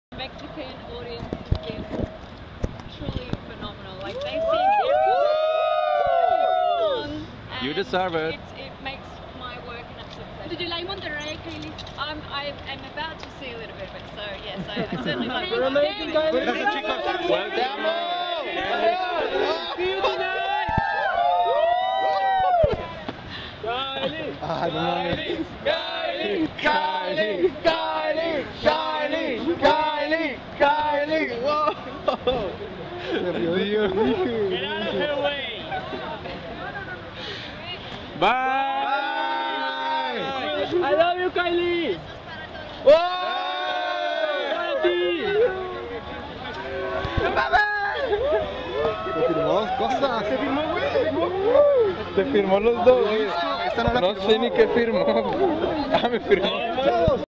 Kylie Minogue @ the hotel in Monterrey @kylieminogue
Sharing some words with the media of Monterrey, México. :D